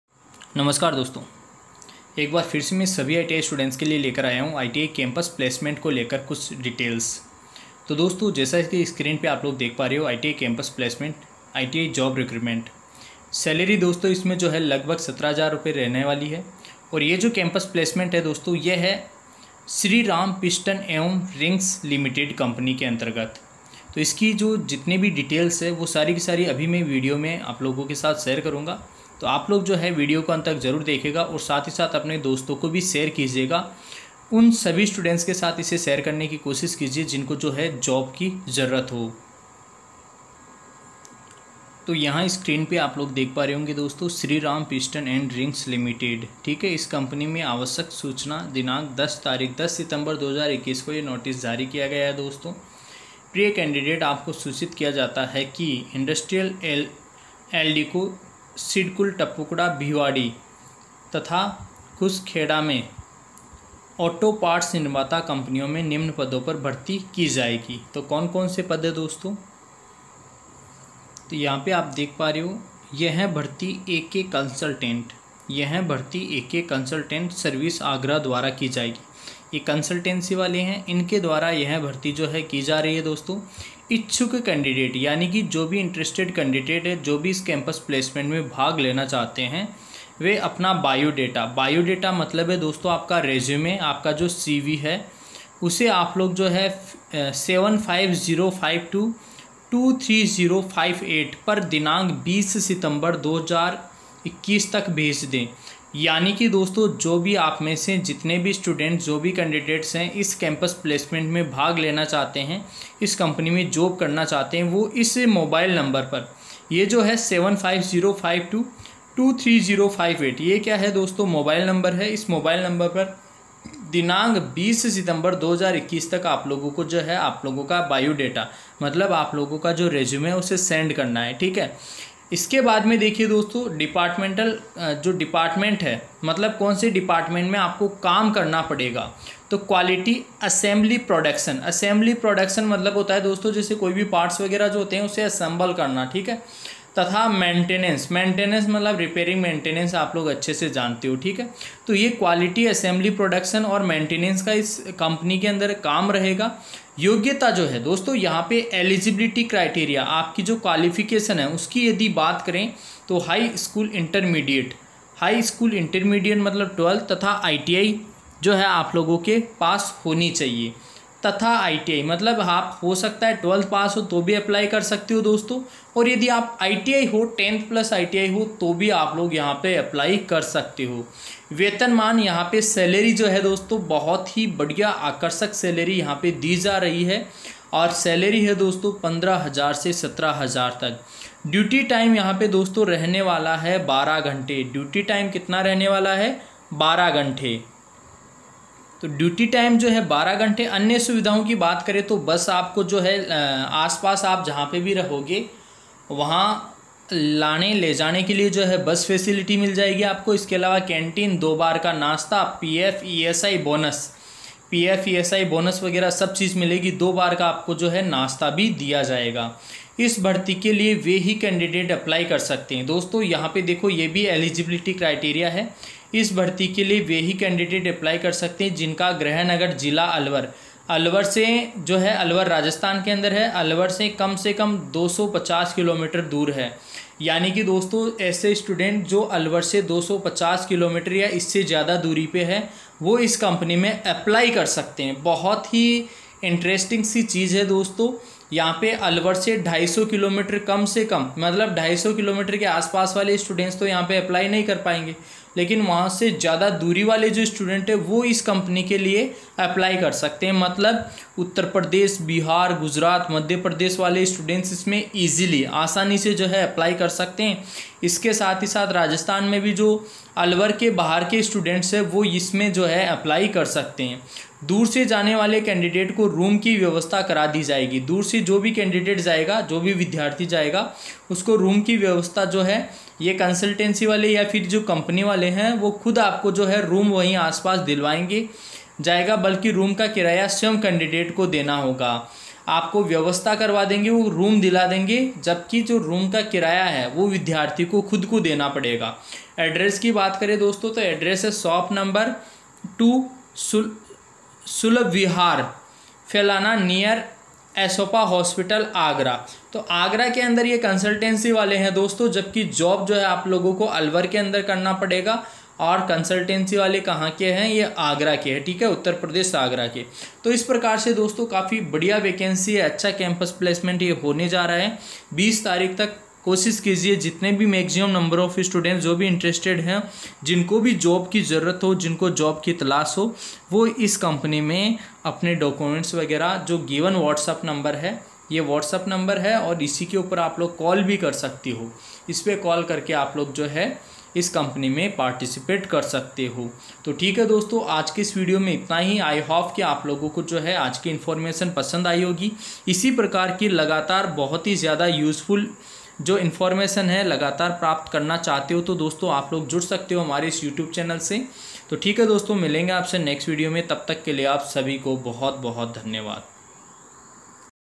ITI जॉब रिक्रूटमेंट | श्रीराम पिस्टन एवं रिंग्स कंपनी | सैलरी 17000/- तक | Last Date 20/09/21
सैलरी_17000/-तक
Last _Date_20/09/21